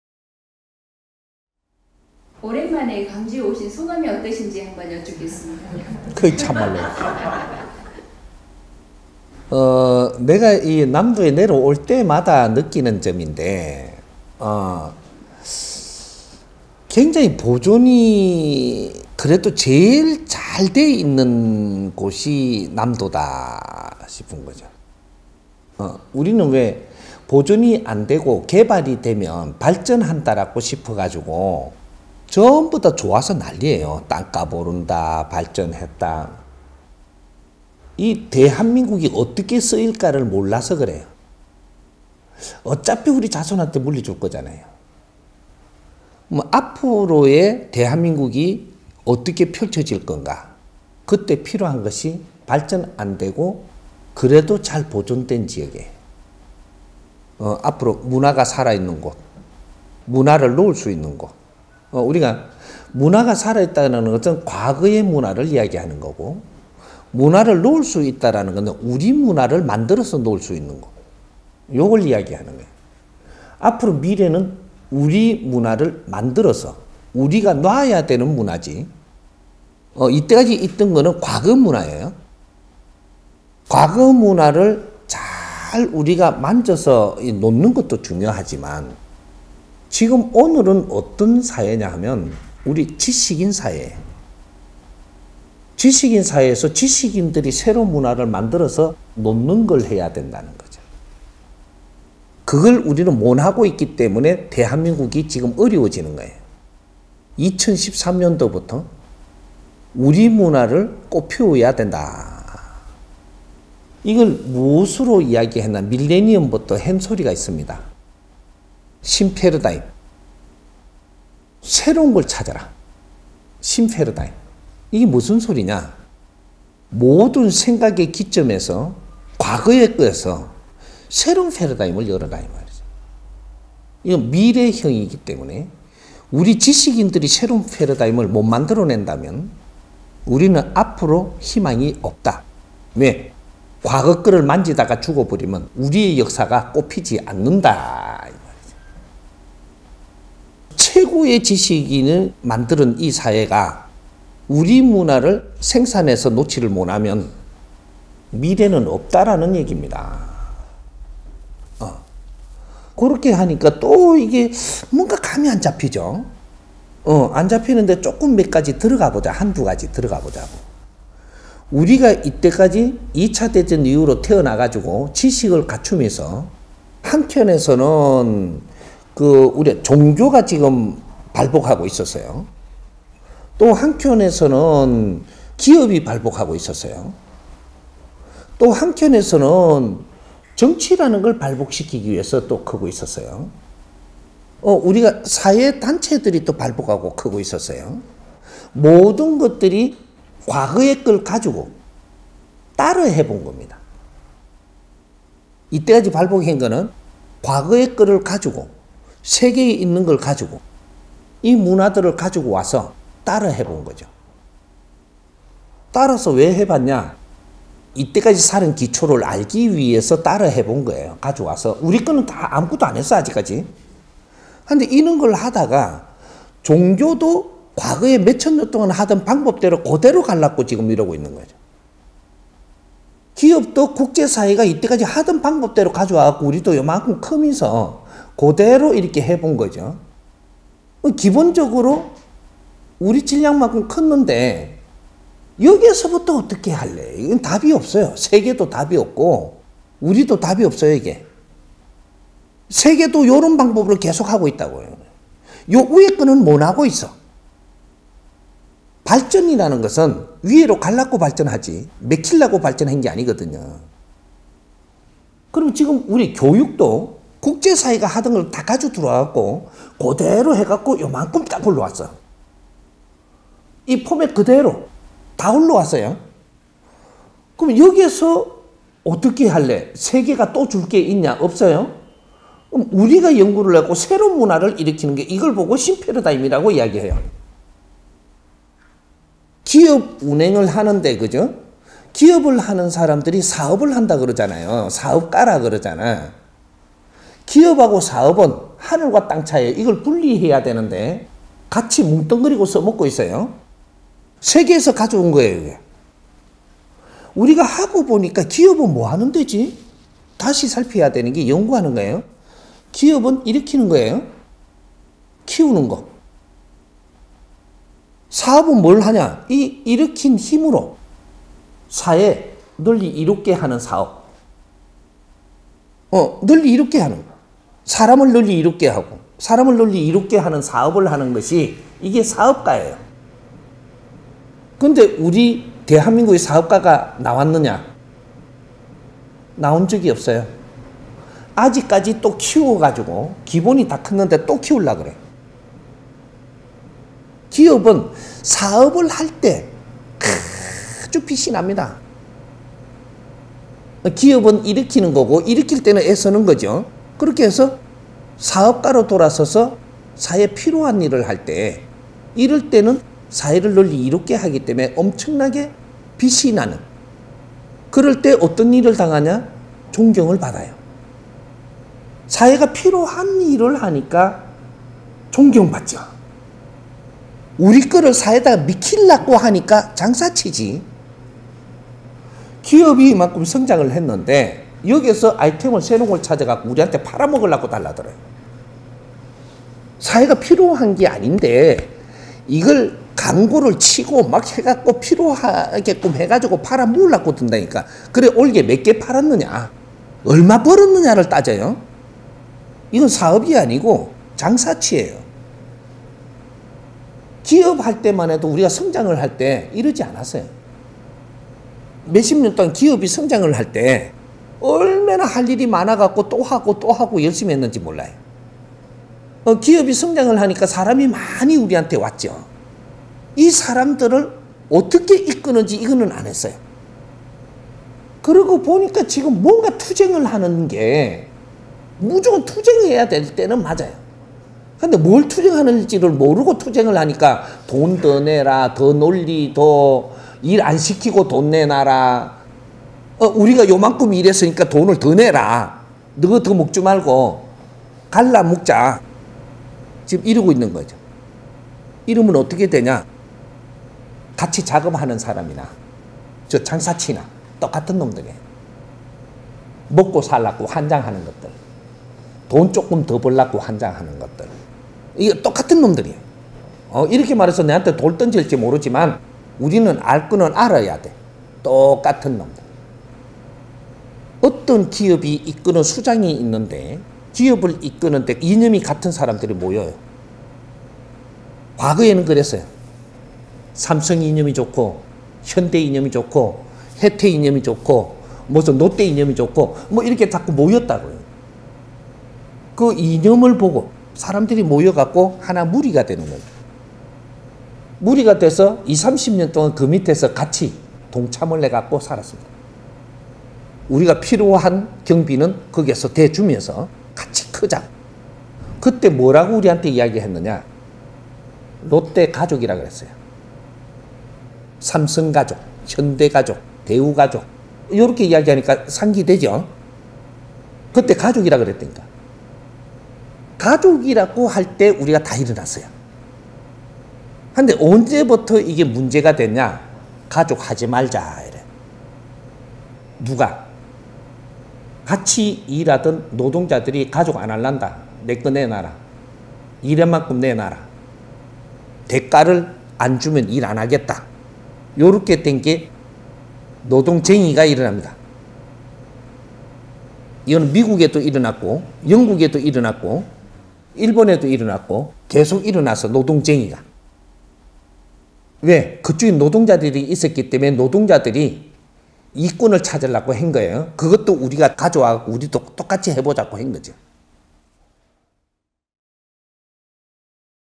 12219강 대한민국의 문화(1_4)[홍익인간 인성교육]
질문: 오랜만에 광주에 오신 소감을 여쭙겠습니다.

강의 일자: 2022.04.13 담양 / 정법시대: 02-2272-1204